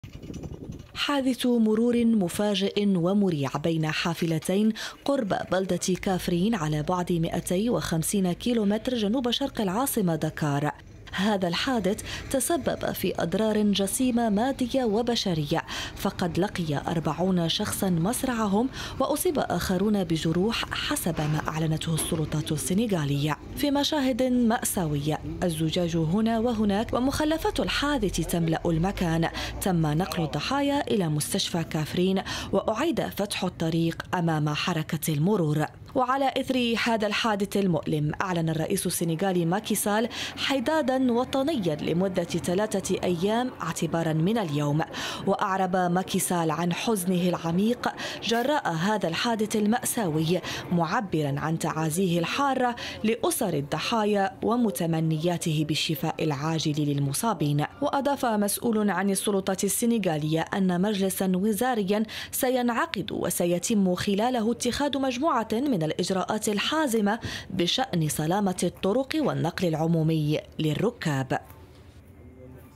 السنغال.. مصرع 40 شخصا في حادثة سير وإعلان الحداد لمدة ثلاثة أيام
لقي 40 شخصا مصرعهم وأصيب آخرون بجروح في حادث تصادم حافلتين قرب بلدة كافرين بوسط السنغال اليوم الأحد، حسبما أعلنته السلطات السنغالية.

لمتابعتنا

لتحميل التطبيق